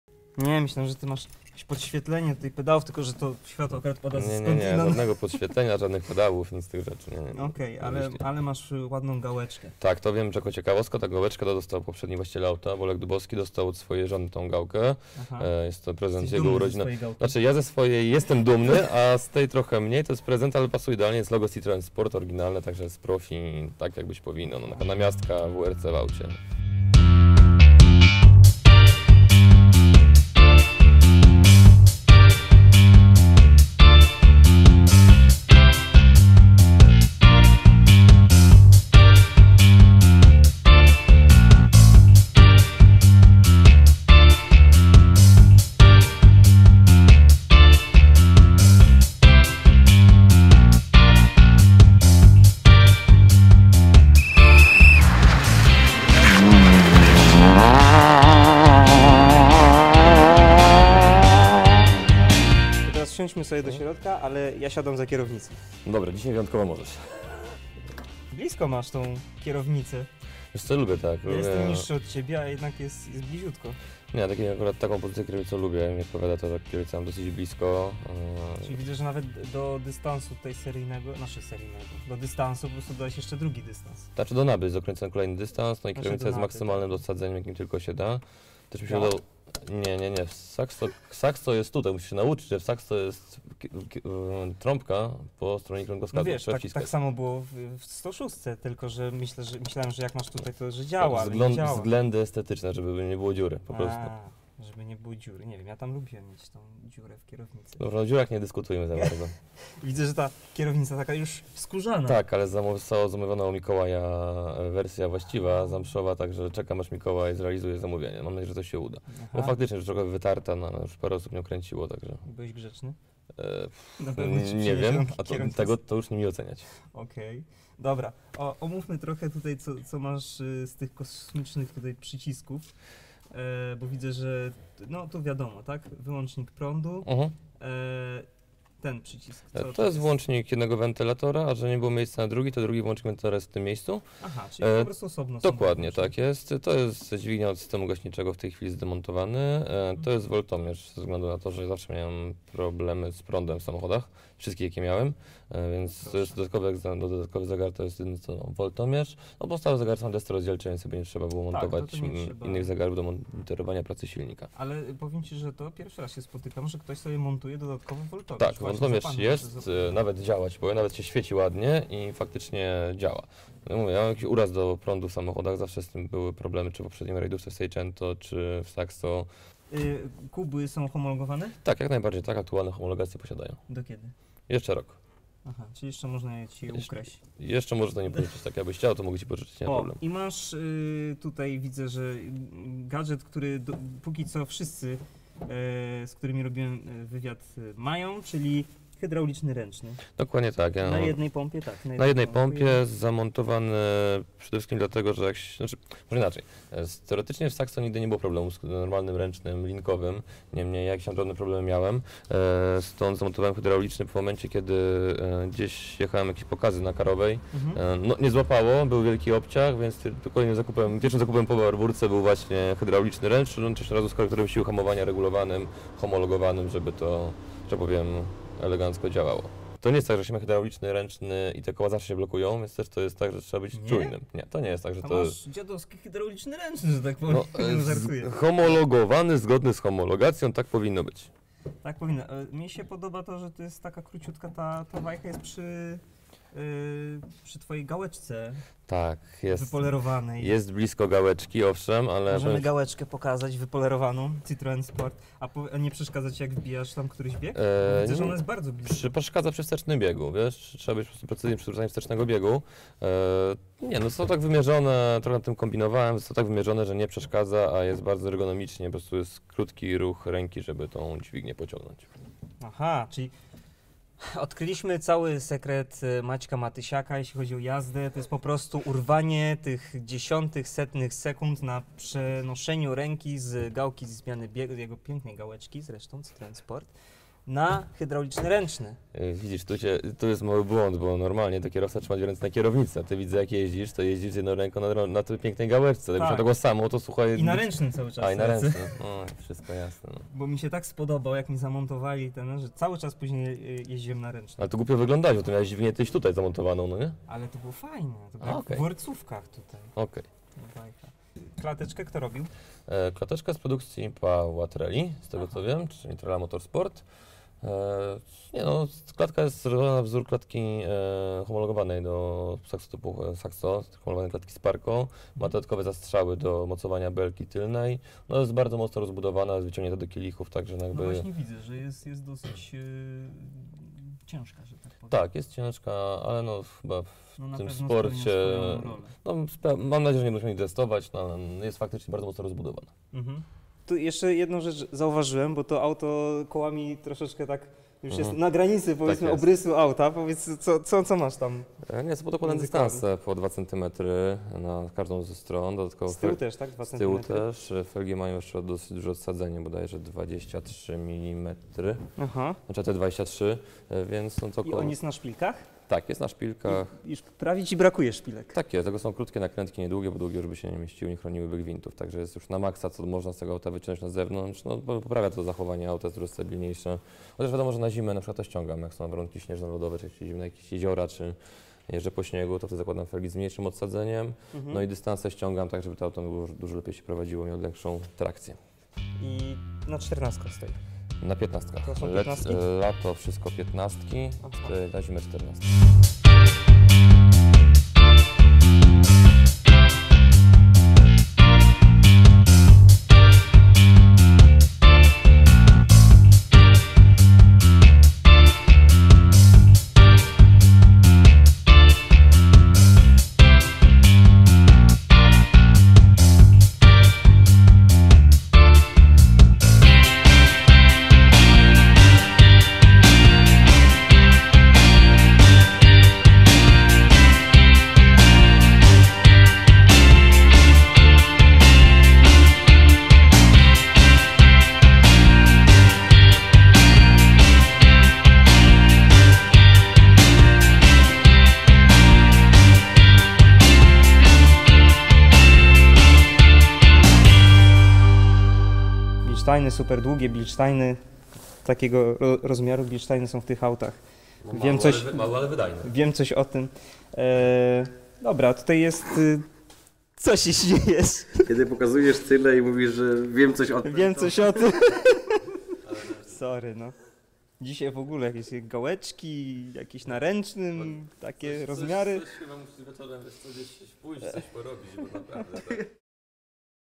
"Garaż" Citroen Saxo część 2
W tym odcinku prezentujemy wam samochód, który wygrał rywalizację w RSMS. Sergio Lisowski prezentuje "typowy" francuski wóz bojowy :)